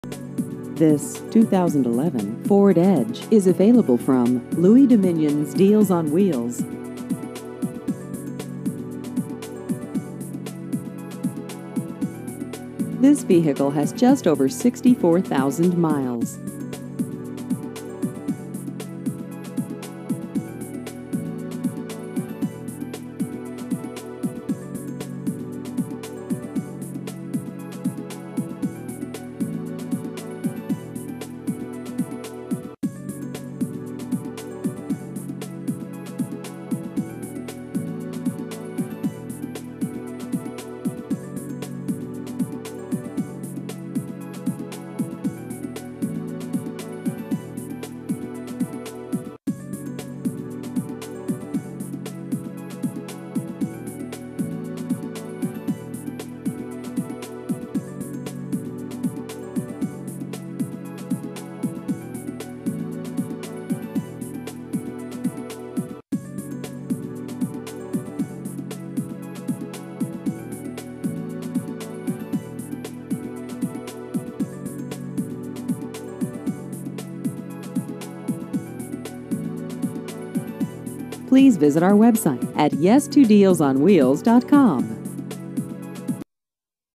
2011 Ford Edge Used Cars Dowagiac MI
This 2011 Ford Edge is available from Louie Dominion's Deals on Wheels.